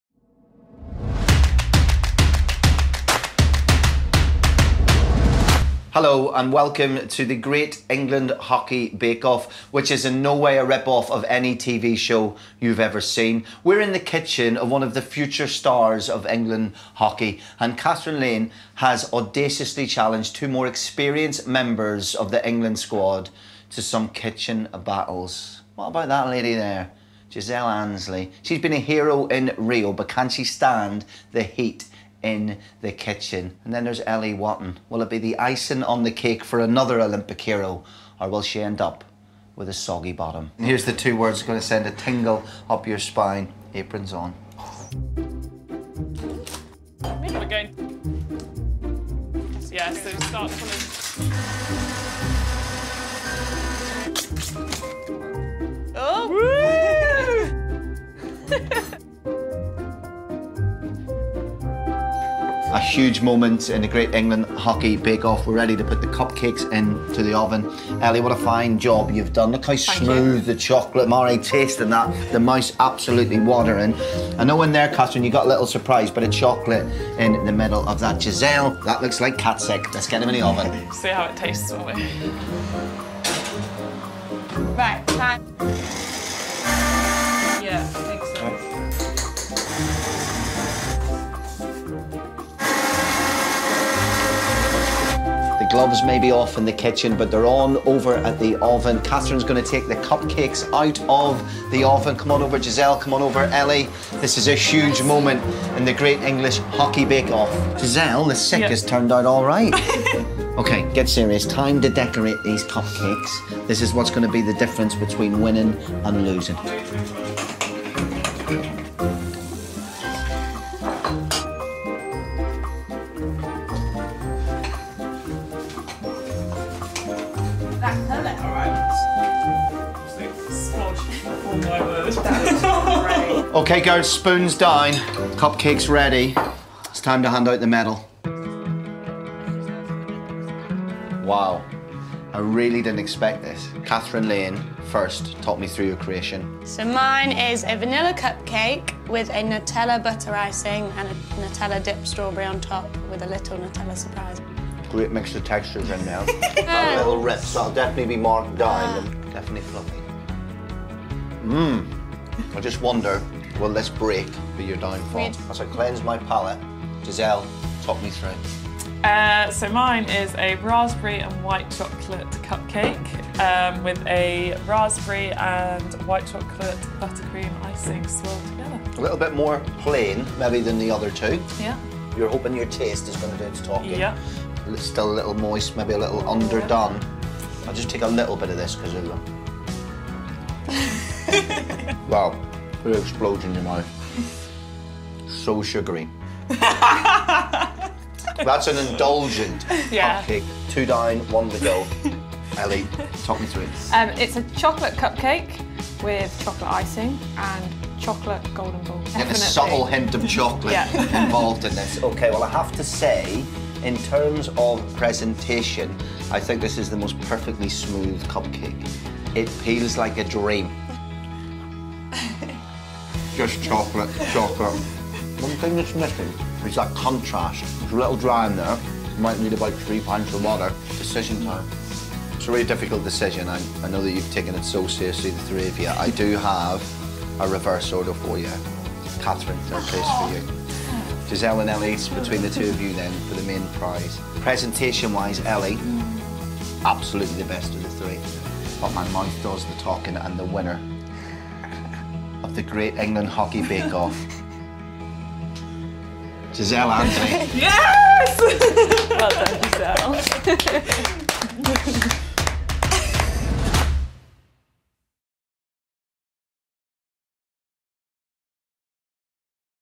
England Hockey Bake Off - Ansley v Lane v Watton! Who Impressed Colin The Most?
They may be good friends on the pitch but the rivalry between England's Giselle Ansley, Kathryn Lane & Ellie Watton was fierce as they battled to see who could make the best cup cakes! But who did Colin name the winner?